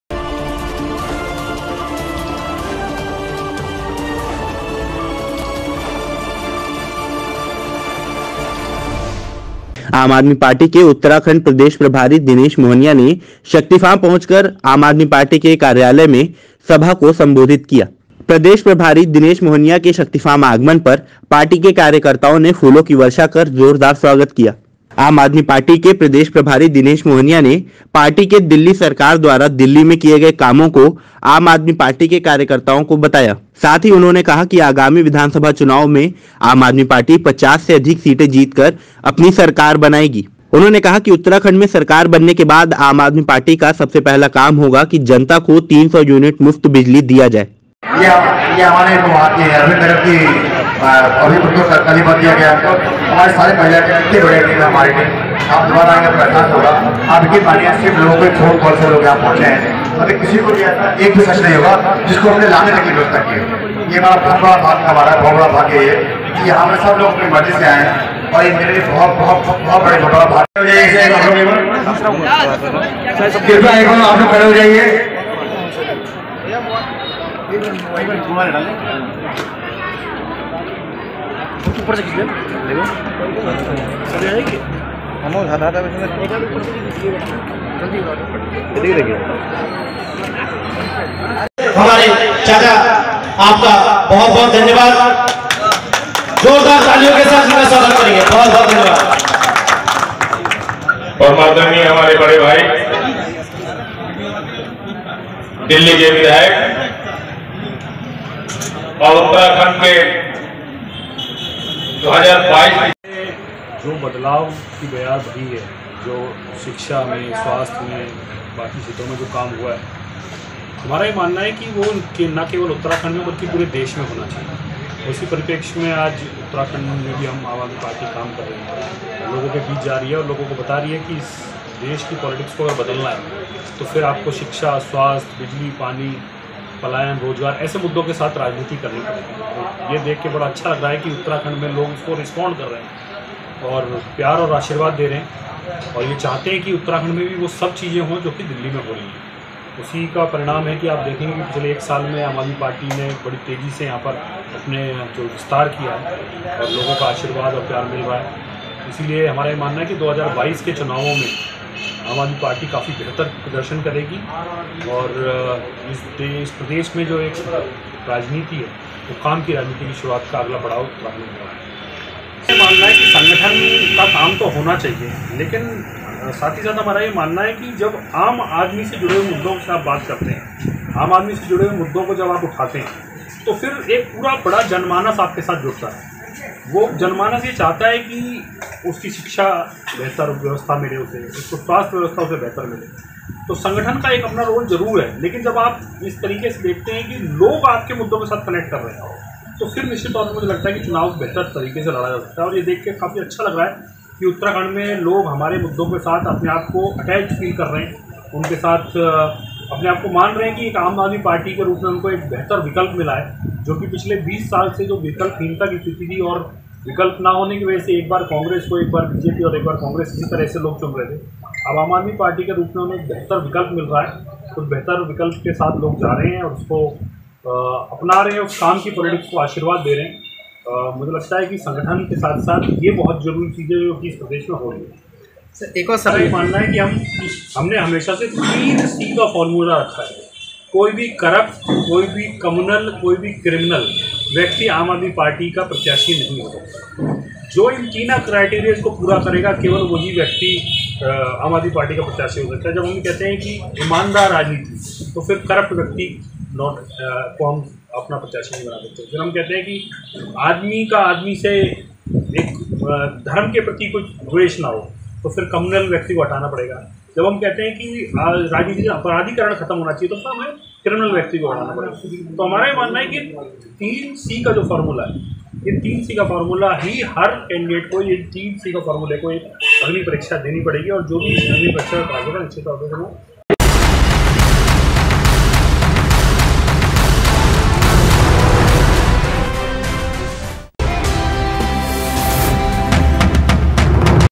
आम आदमी पार्टी के प्रदेश प्रभारी दिनेश मोहनिया ने किया प्रेस कॉन्फ्रेंस || T NEWS LIVE 24X7
आम आदमी पार्टी के प्रदेश प्रभारी दिनेश मोहनिया ने शक्तिफार्म पहुंचकर आम आदमी पार्टी के कार्यालय मैं सभा को संबोधित किया और जोरदार स्वागत  किया। वही आम आदमी पार्टी के प्रदेश प्रभारी दिनेश मोहनिया शक्तिफार्म आगमन पर पार्टी कार्यकर्ताओं ने फूलों की वर्षा कर जोरदार स्वागत किया। साथ ही पार्टी कार्यकर्ताओं के साथ बैठक की।
 वही आम आदमी पार्टी के प्रदेश प्रभारी दिनेश मोहनिया ने आम आदमी पार्टी के दिल्ली सरकार द्वारा दिल्ली में किये गए कामो को आम आदमी पार्टी के कार्येकर्ताओ को बताया। साथ ही पार्टी कार्यकर्ताओं में आगामी 2022 के चुनाव को लेकर जोश भरा।कहा कि आगामी 2022 के चुनाव में पार्टी 50 प्लस सीटे जीतकर विधानसभा जाएगी और आगामी 2022 में आम आदमी पार्टी की सरकार उत्तराखंड में बनेगी। प्रदेश प्रभारी ने कहा कि 2022 में आम आदमी पार्टी की उत्तराखंड में सरकार बनने के बाद उत्तराखंड की जनता को तीन सौ मिनट मुफ्त बिजली देने का आम आदमी पार्टी के सुप्रीमो अरविंद केजरीवाल ने ऐलान किया है जो उत्तराखंड में सरकार बनने के बाद सबसे पहले मुफ्त बिजली देने का काम होगा।